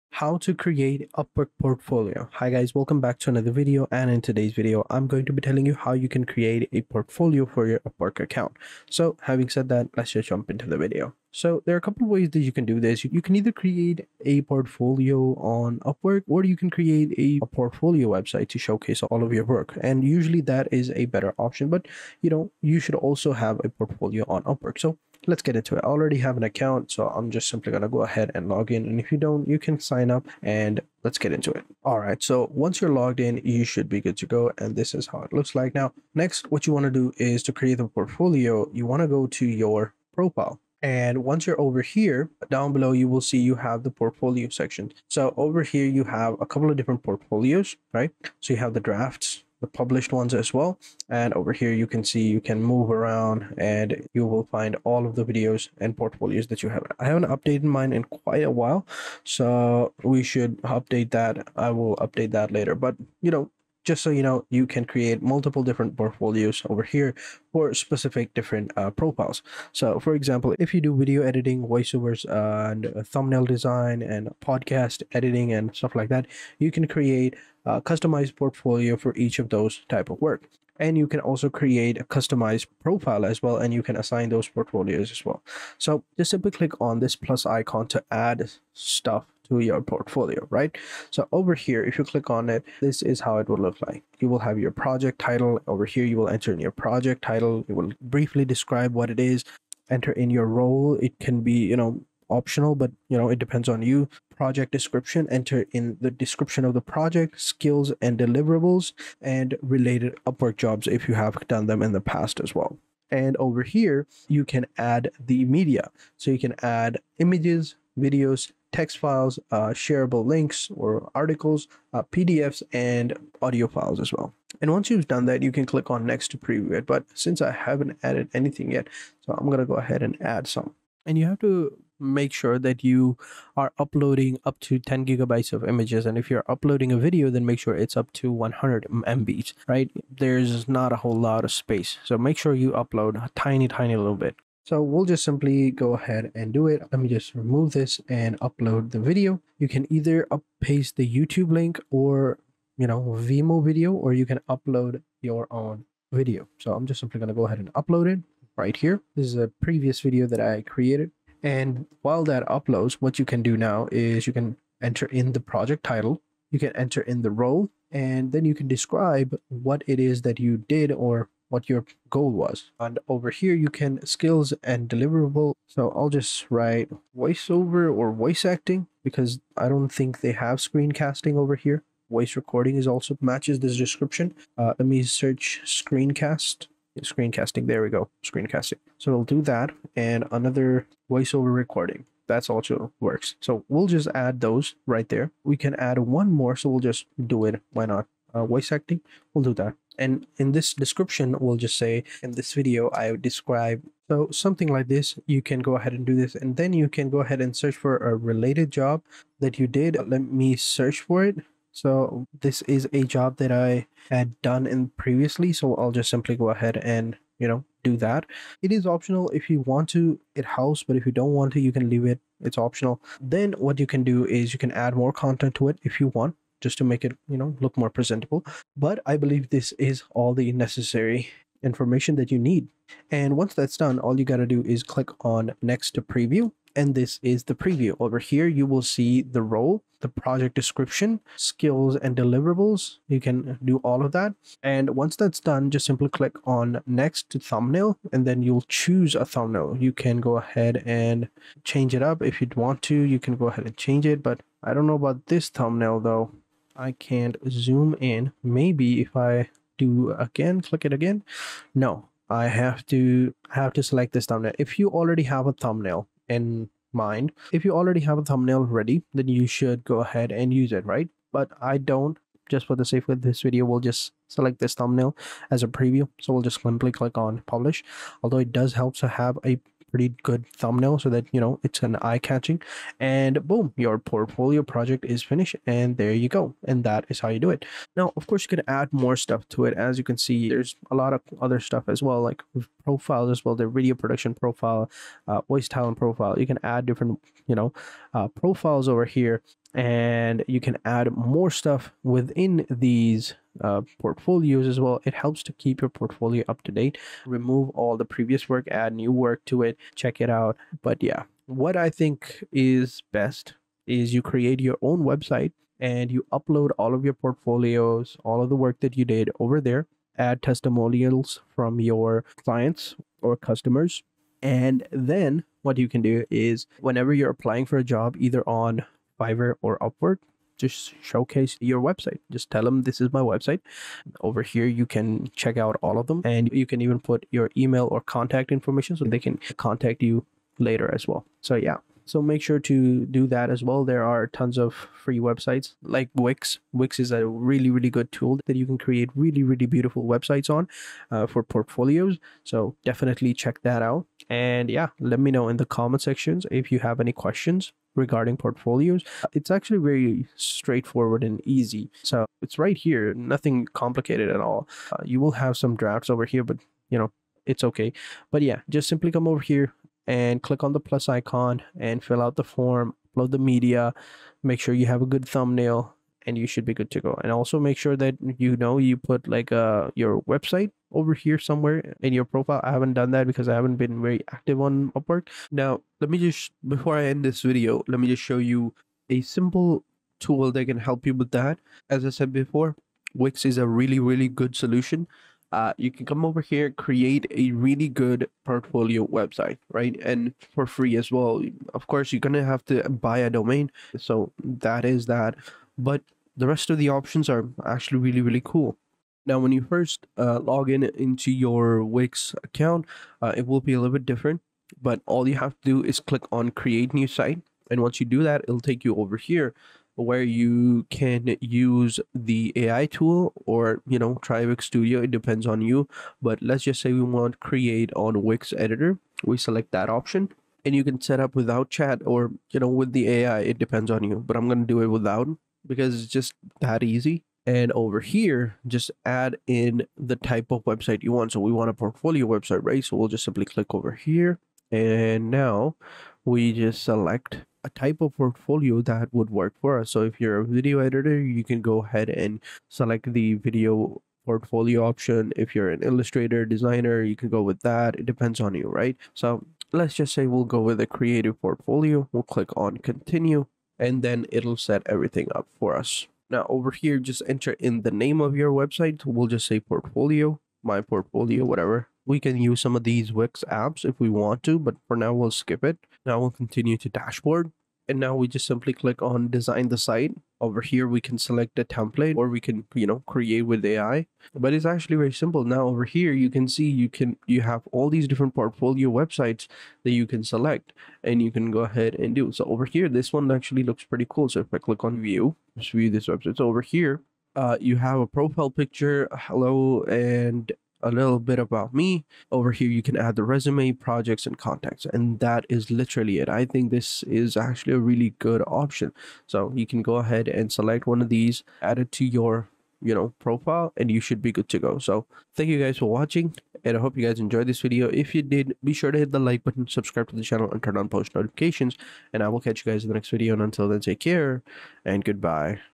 How to Create Upwork Portfolio 2026 (Step by Step)
How to Create Upwork Portfolio

In this video, I show you how to create a portfolio on Upwork! This video will help you in getting started on upwork. Thank you so much!